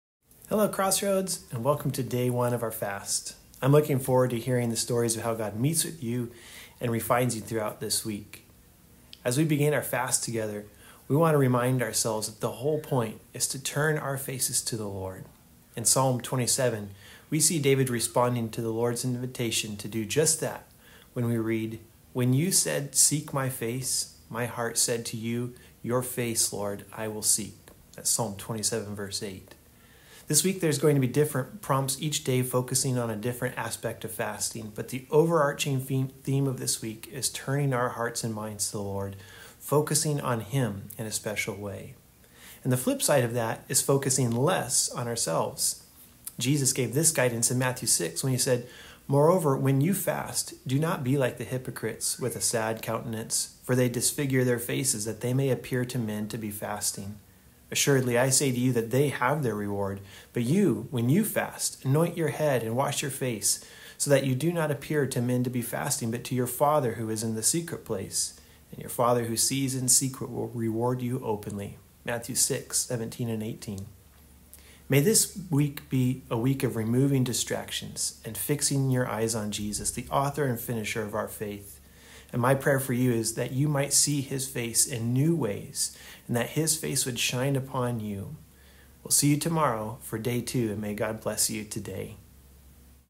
Week of Fasting, Day One
Day One
Turning Our Hearts to the Lord

As we begin our fast together, we want to remind ourselves that the whole point is to turn our faces toward the Lord. In Psalm 27, we see David responding to the Lord’s invitation to do just that when we read, “When You said, ‘Seek My face,’ My heart said to You, ‘Your face, Lord, I will seek.’” (Psalm 27:8)

This week, there will be prompts each day focusing on different aspects of fasting, but the overarching theme of this week is turning our hearts and minds to the Lord, especially focusing on Him.

And the flip side of that is focusing less on ourselves. Jesus gave this guidance in Matthew 6 when He said, “Moreover, when you fast, do not be like the hypocrites, with a sad countenance. For they disfigure their faces that they may appear to men to be fasting. Assuredly, I say to you, they have their reward. But you, when you fast, anoint your head and wash your face, so that you do not appear to men to be fasting, but to your Father who is in the secret place; and your Father who sees in secret will reward you openly.” (Matthew 6:17-18)

May this be a week of removing distractions and fixing your eyes on Jesus, the author and finisher of our faith. May you see His face in new ways and may His face might shine upon you.